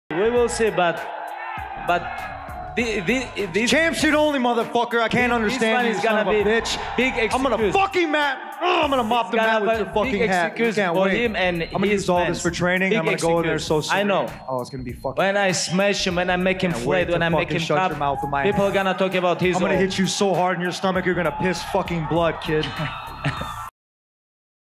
Tony Ferguson Vs Khabib Crazy Trash Talk At UFC 249 Press Conference
Tony Vs Khabib Crazy Trash Talk At UFC 249 Press Conference